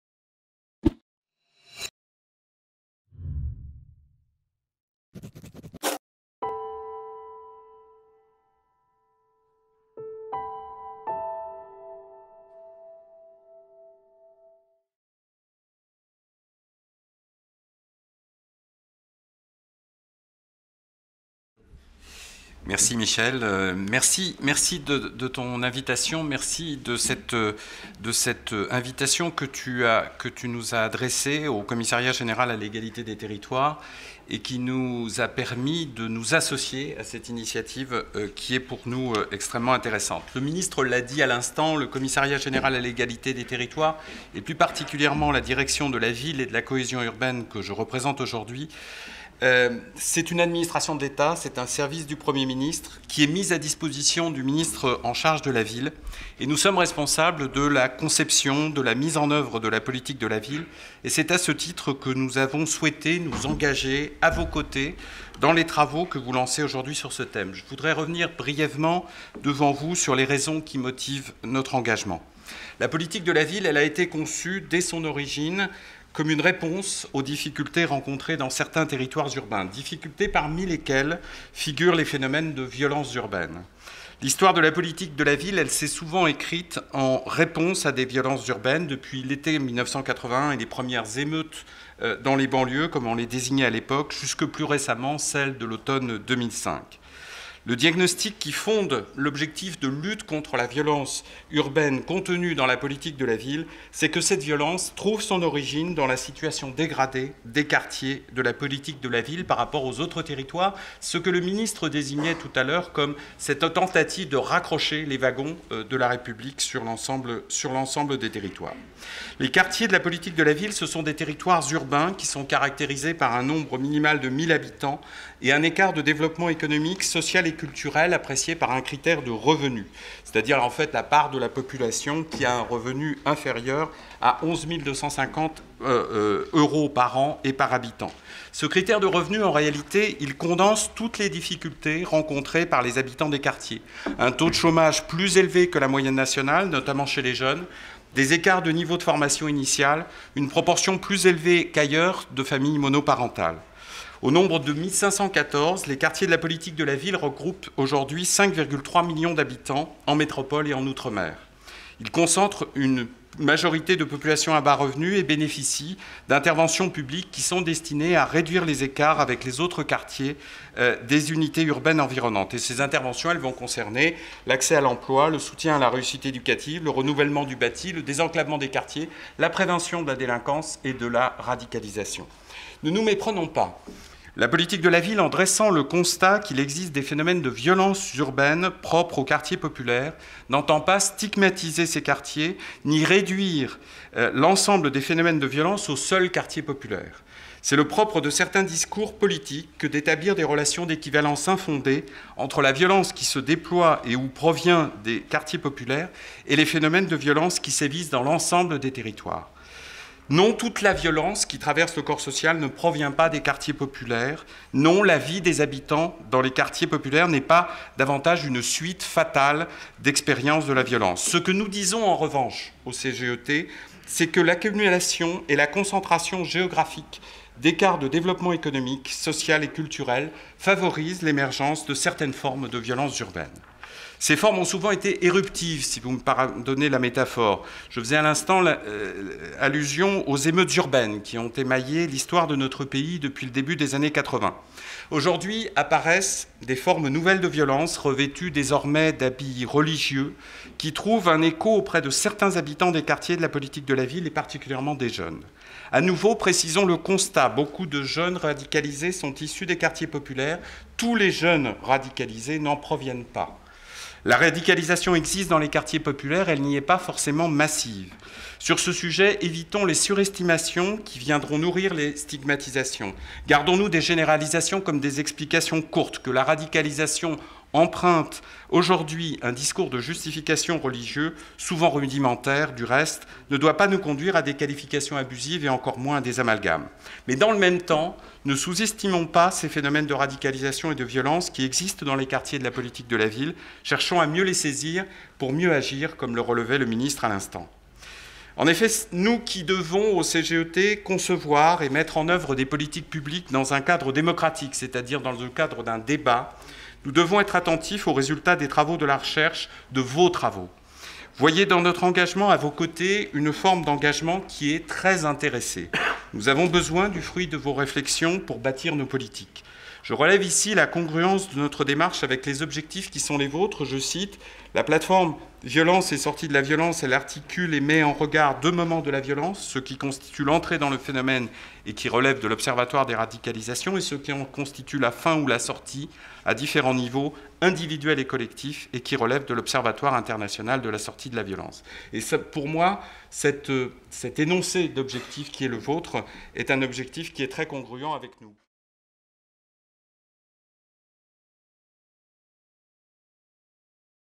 Eric Briat, CGET: "We need the fruits of your reflections to build or policies"
Eric Briat, from Commissariat Général à l'Egalité des Territoires (CGET) presents the value of the project during the IPEV Kick-Off.